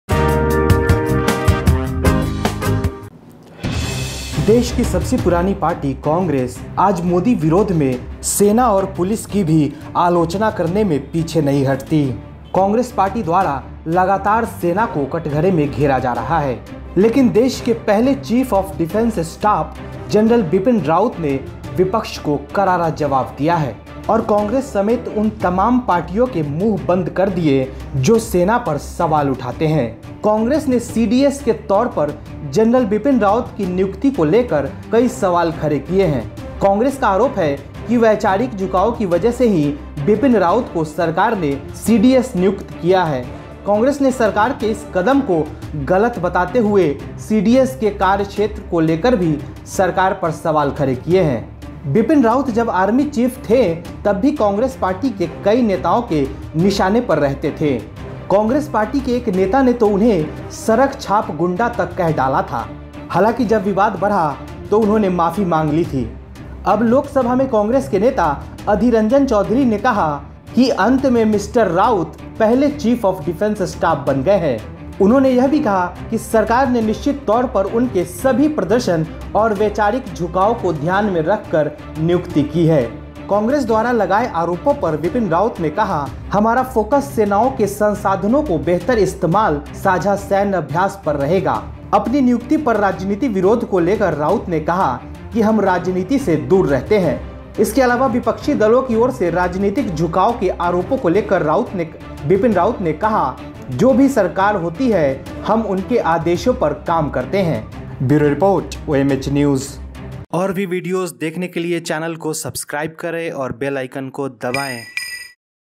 CDS Bipin Rawat's great reply to Congress
The Congress on Tuesday raised several questions about the appointment of General Bipin Rawat as Chief of Defense Staff (CDS). The Congress alleges that Bipin Rawat has been appointed CDS by the government due to 'ideological leanings'. Describing the government's move as wrong, the party has also asked the government questions regarding the scope of CDS. Congress leader Adhir Ranjan Chowdhury's many questions from Modi goverment for the CDS appointment.